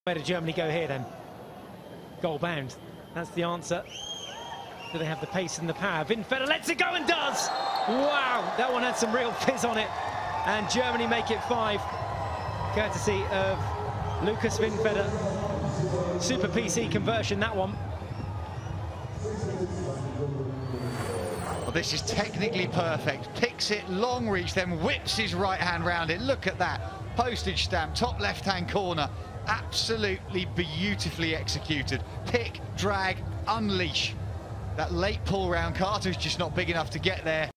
GER 5-0 CAN Winfeder flicks its perectly into the top corner
Watch clips from the Germany - Canada game at the FIH 2015 Men’s Hero Hockey World League, taking place in Raipur, India.

The FIH Men’s Hero Hockey World League Finals take place from November 27th-December 6th, 2015, see the full schedule or check results here: